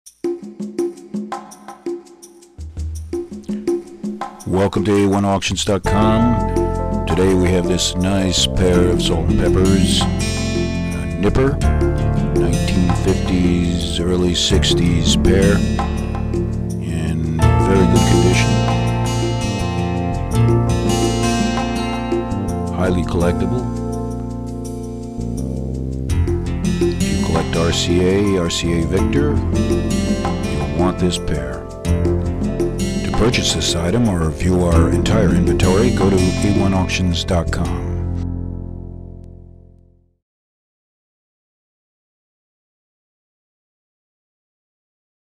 1950's Nipper the Dog Salt & Pepper For Sale
-this salt and pepper shaker set is perfect for the collector of music memorabilia. RCA Victor VG condition. Go to AntiqueWeb.com or A1Auctions .com for more information or to view our complete antiques and collectibles inventory.